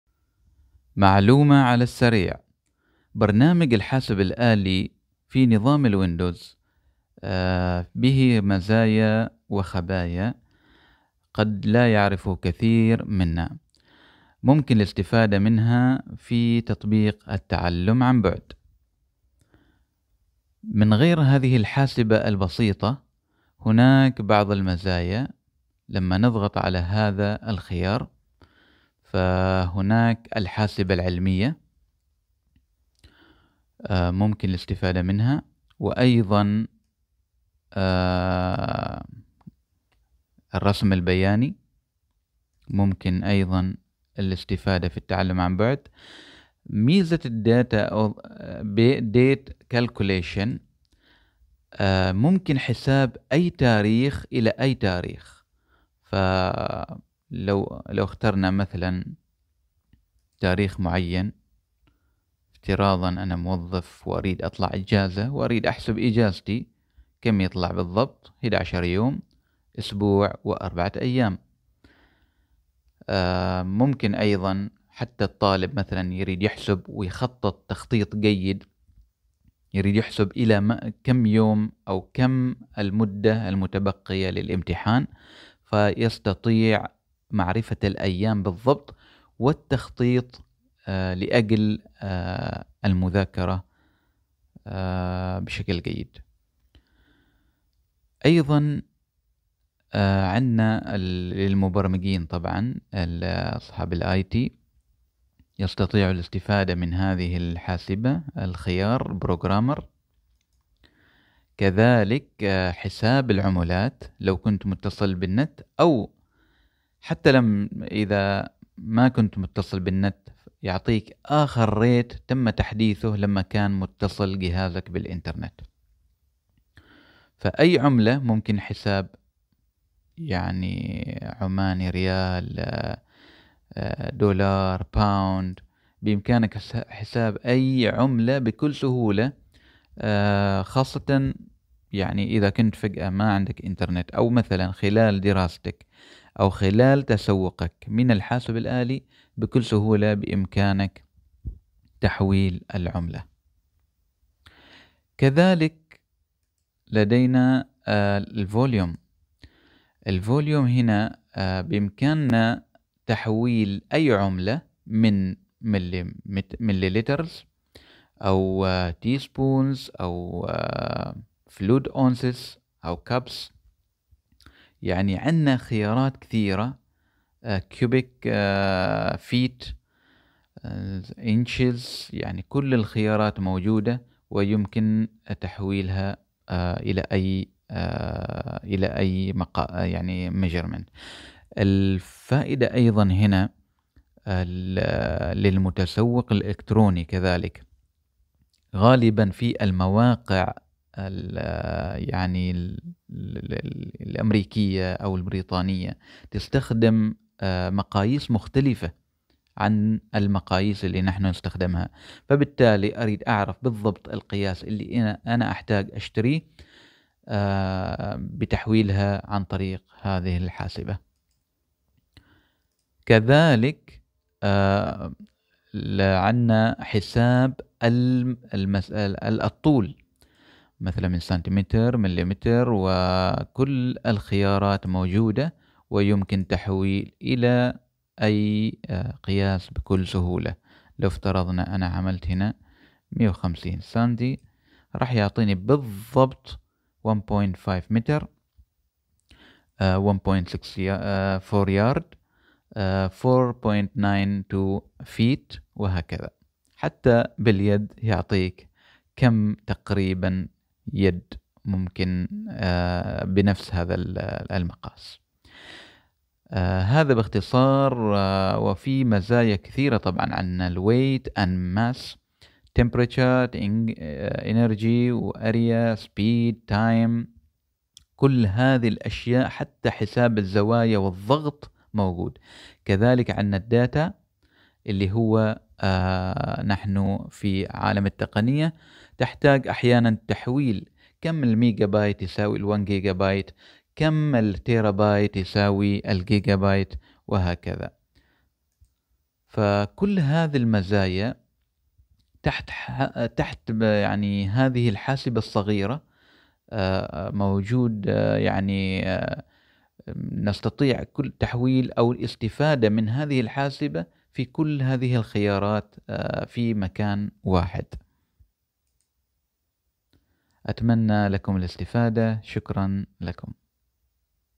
مزايا قد لا تعرفها في برنامج الحاسبة البسيطة | مفيدة للمعلم والطالب في تطبيق التعلم عن بعد
برنامج الحاسبة في مايكروسوفت ويندوز بها مزايا قد لا تعرفها ، تابع معنا حتى تتعرف عليها